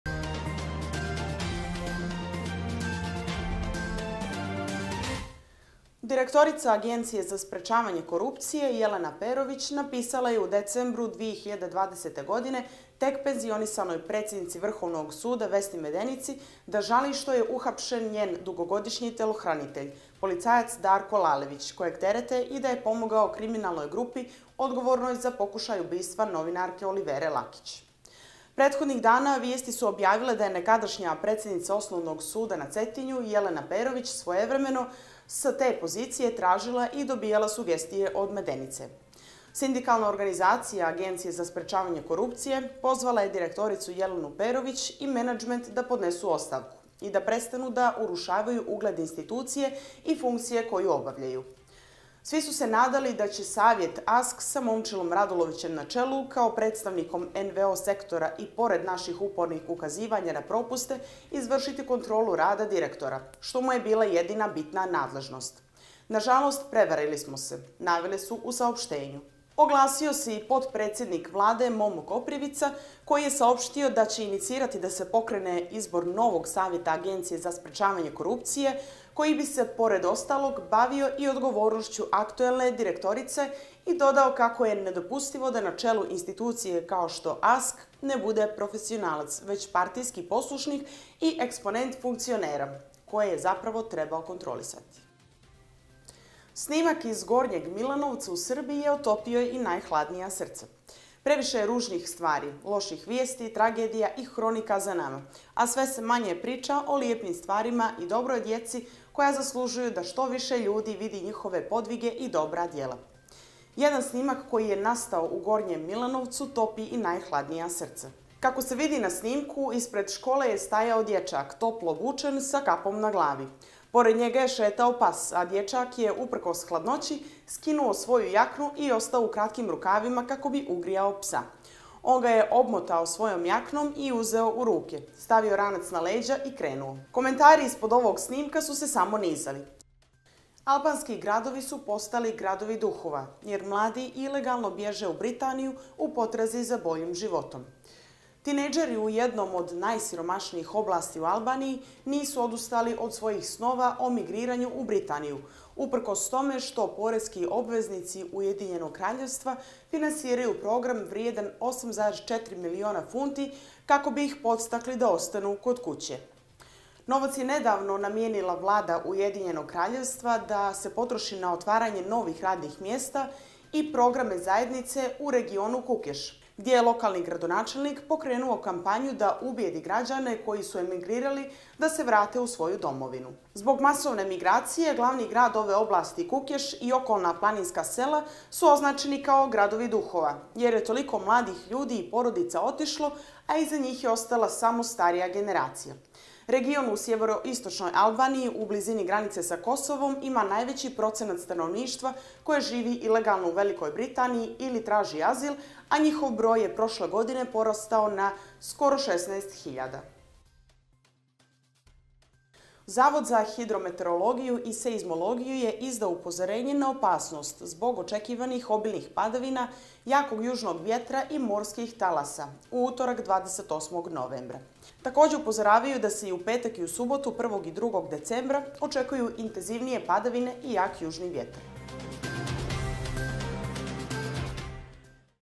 SNIMAK DJEČAKA I PSA ODUŠEVIO REGION! "GRADOVI DUHOVA" U ALBANIJI!
Donosimo vam naslove dana, vijesti iz regiona i svijeta, sporta i kulture! Pratite MONDO VIJESTI!